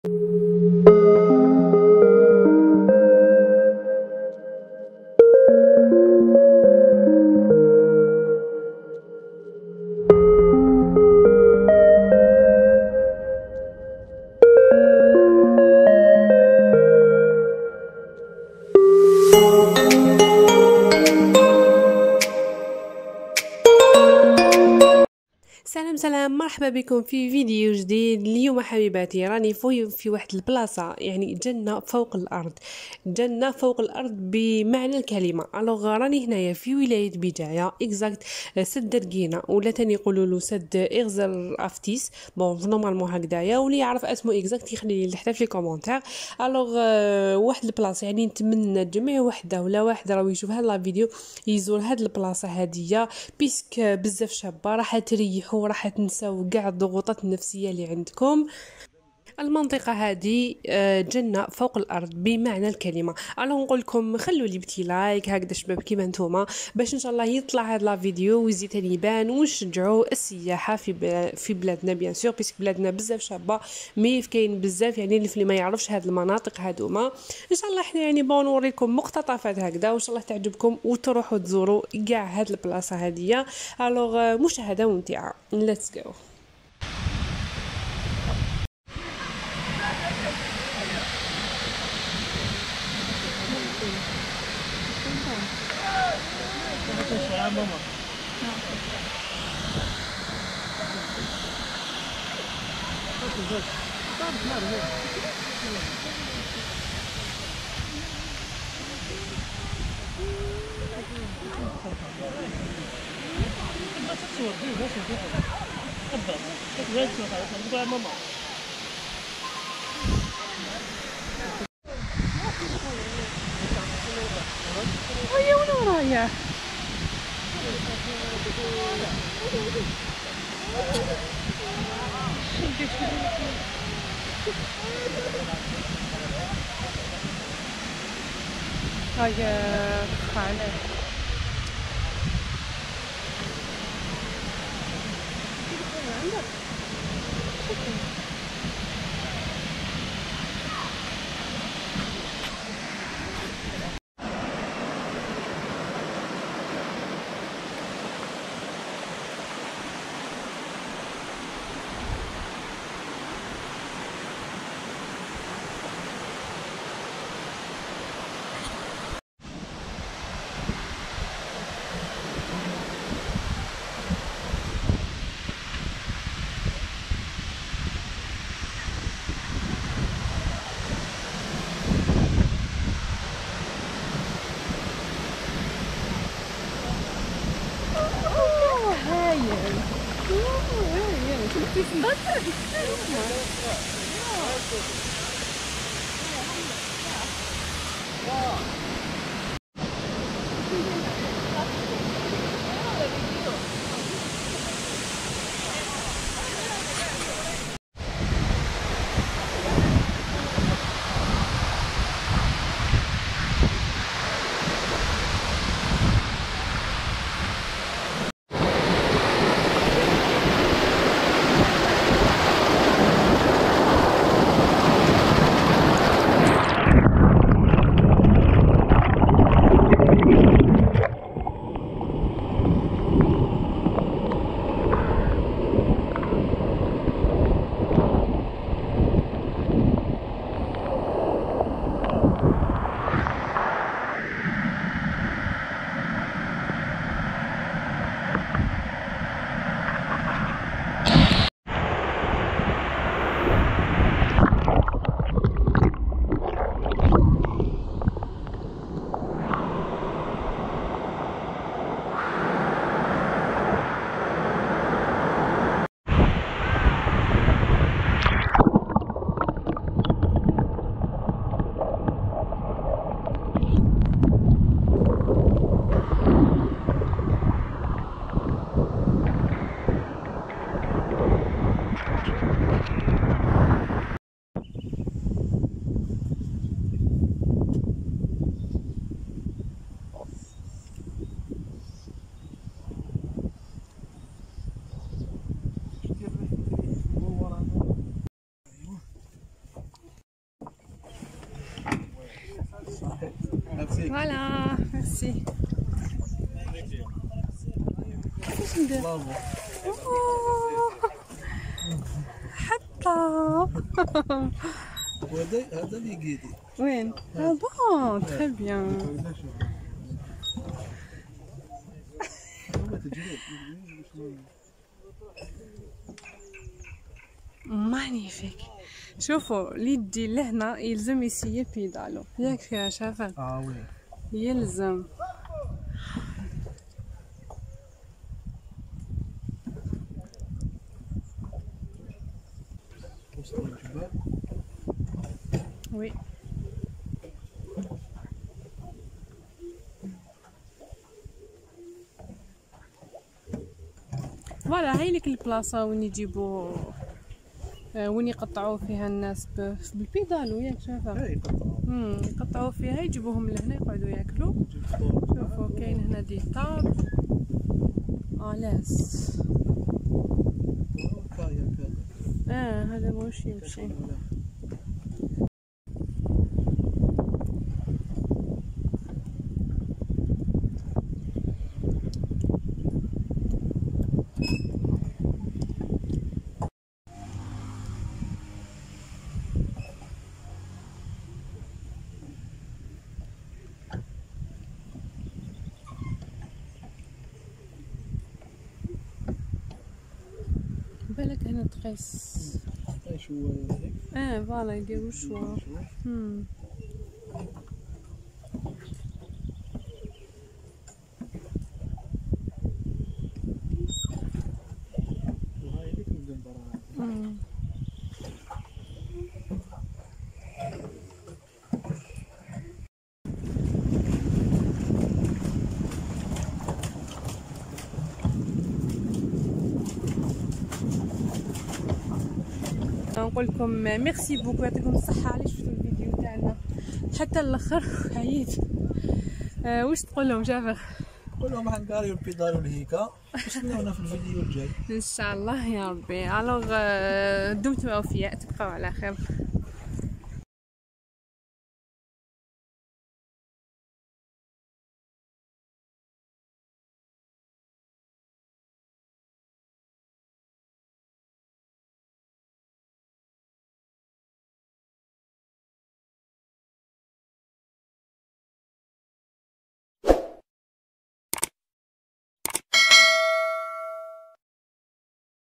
.سد درڨينة ❤"اغزر افتيس" من أروع السدود في الجزائر 🇩🇿, بجاية😍
سد إغزر أوفتيس أو سد درقينة هو سد مائي من الخرسانة بطول 2 كـم (1 ميل) يقع في قرية إغزر أوفتيس في وسط بلدية درقينة قرب محطة كهرباء درقينة على وادي إغزر أوفتيس في دائرة درقينة بولاية بجاية في الجزائر. شيد ما بين 24 أوت 1945م و26 فيفري 1950م، ويستعمل أساسا في توفير ماء الشرب وماء الري.